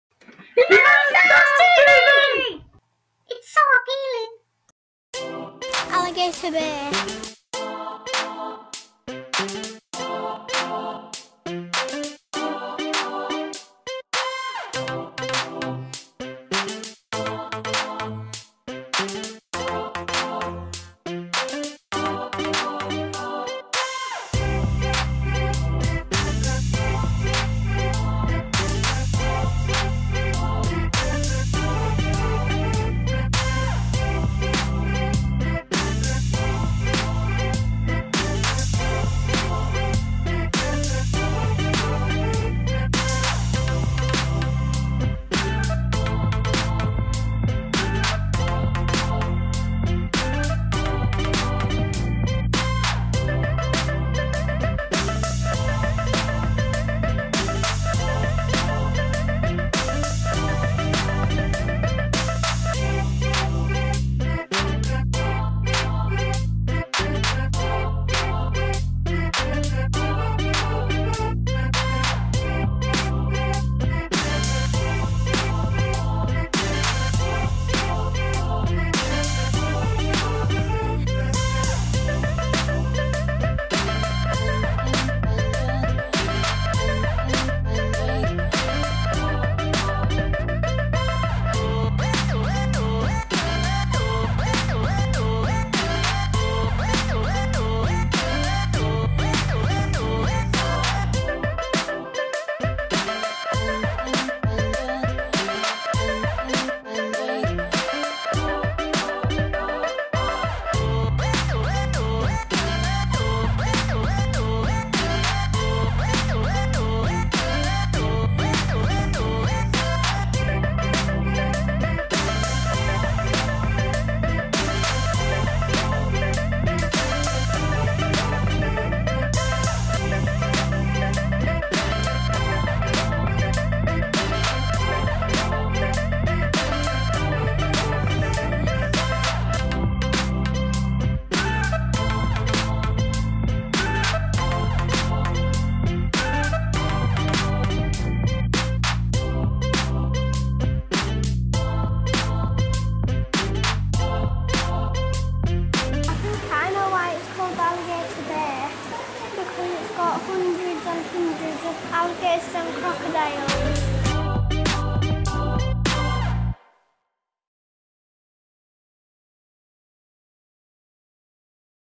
Will the Alligator Bite Amelia's Head off?
Alligator Bay is one of the nicest little zoo's we have ever visited. Hands on fun with the Turtles, lots to see & do for the children and many, many reptiles including alligators, crocodiles, snakes and lizards.

Visited...
Alligator Bay in Normandy, France
Located at Mont st Michel

If you have any requests for future videos please get in touch. We would love to hear from you!